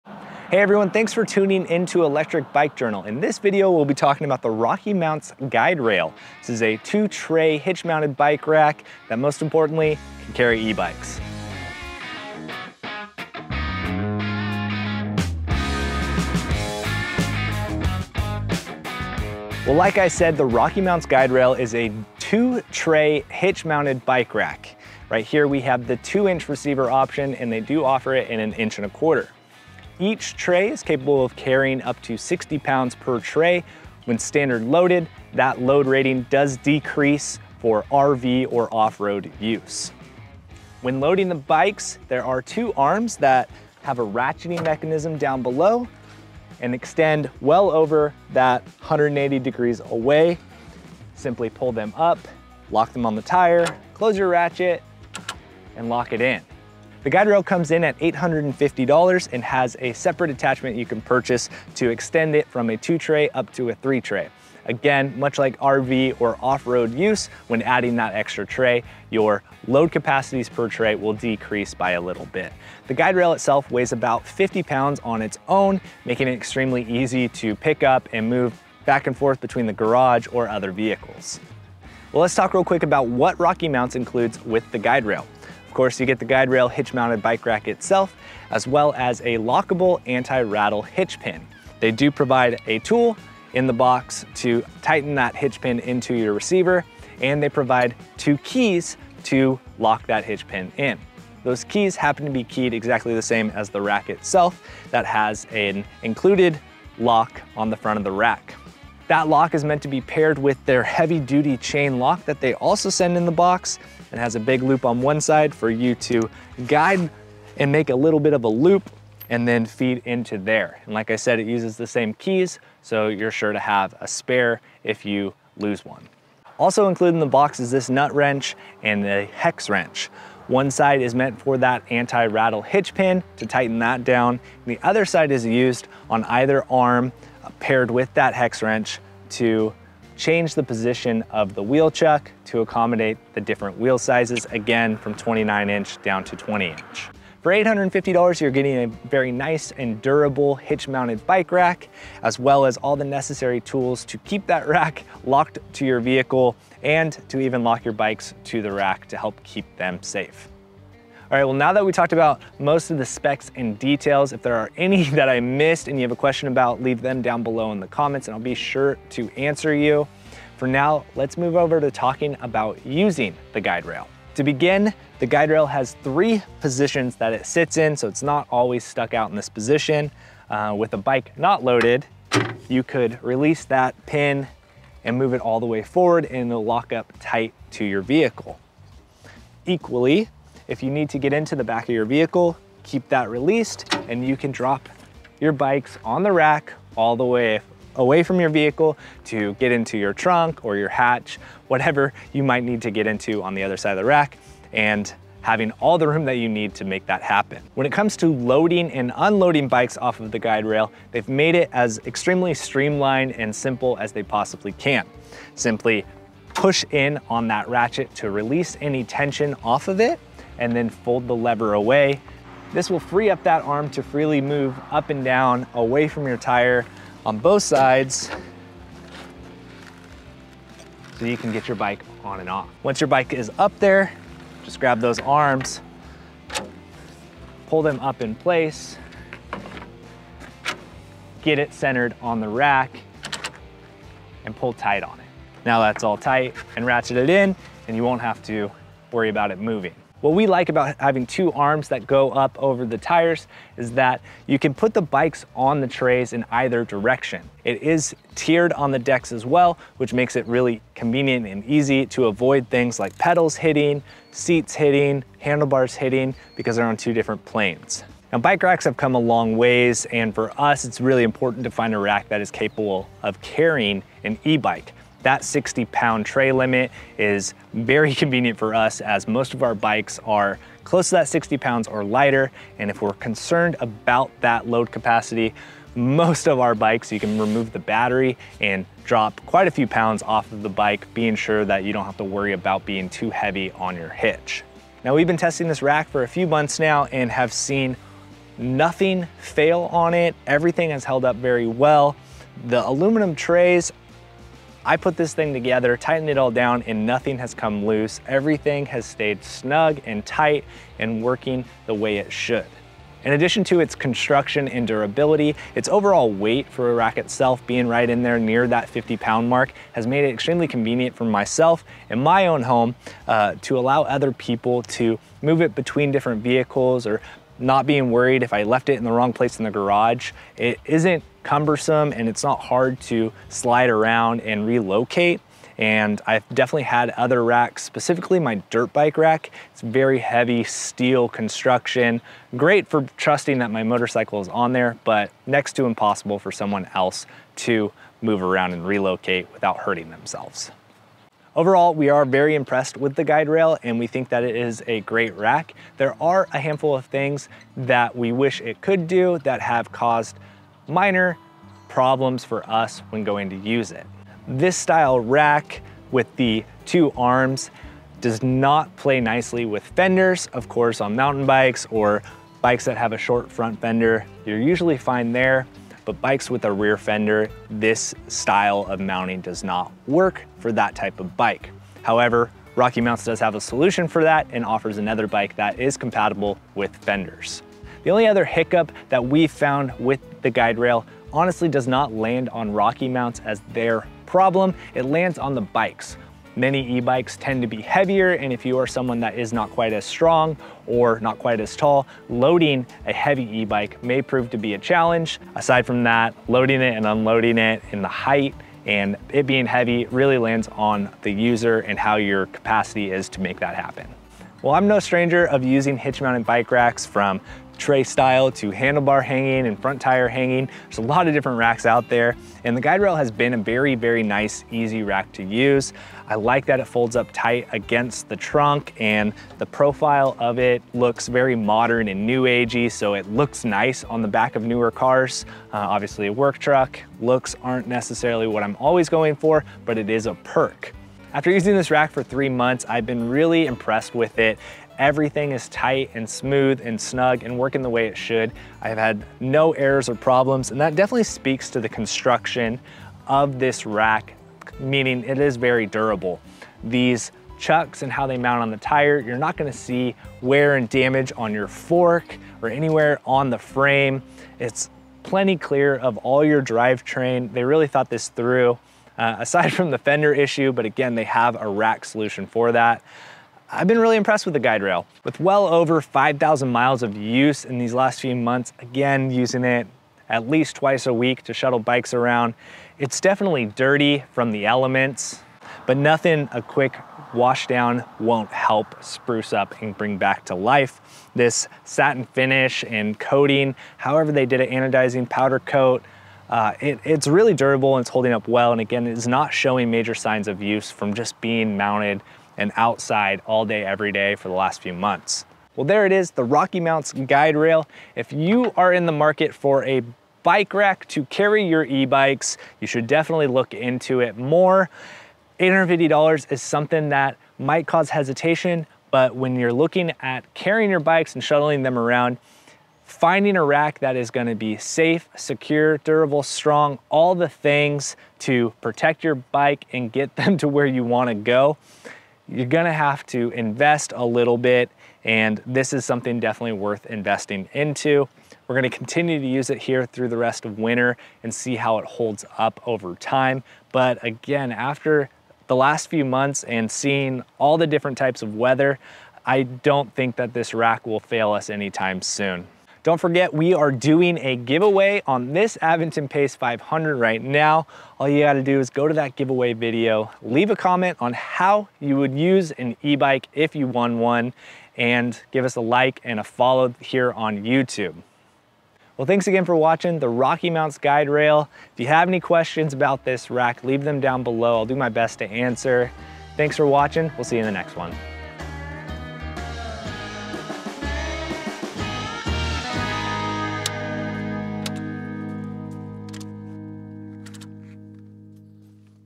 eBike Rated RockyMounts GuideRail Bike Rack Review
The RockyMounts GuideRail is hands down one of the nicest racks we've used to date. For $849.99 you get a lightweight, stout, aluminum bike rack that looks very modern and nice on the back of your car or truck. That's just the visual perk, functionally speaking this rack is capable of carrying two bikes in its original configuration up to 60lbs per tray. There is a tray add on for $299 to make it a three bike carrier with a minor decrease in load capacity per tray, much the same as a decrease in load capacity for rv or offroad use. The two arm design with the ratchet mechanism locks the bikes in easily and snug. Additionally you have the freedom to load bikes facing either direction, so no need to worry about which way you started to load your bike. They don't bounce around or move at all once locked in. Equally by pushing the blue lever in before flicking it out, you release the pressure on the ratchet making it extremely easy to release the arms and extend them out of the way when unloading the bikes. This little detail may not seem like much but after fighting with various other arm mechanism's and having to squeeze the release with all your might, this light touch pressure release is the solution you didn't know you were looking for.

For our use, we have found the RockyMounts Guiderail to be built to a very high standard and after thousands of miles and many months of testing we don't forsee this rack to let us down anytime in the future. The Guiderail has one major limitation when it comes to its rather wide variety of bike sizes it can accommodate, and that is it's inability to secure bikes outfitted with fenders. Although a problem for the Guiderail, RockyMounts offers other racks suitable for that task and they all meet the same build quality and carry over most of the same features seen on the Guiderail.

CHAPTERS:
Intro - 0:00
What's Included - 1:21
Rack Positions - 2:54
Loading and Unloading - 3:27
Bike Positioning - 4:11
Load Capacity - 4:28
Limitations - 5:51
Final thoughts - 6:54
BIKE GIVEAWAY - 9:12
Outro - 9:28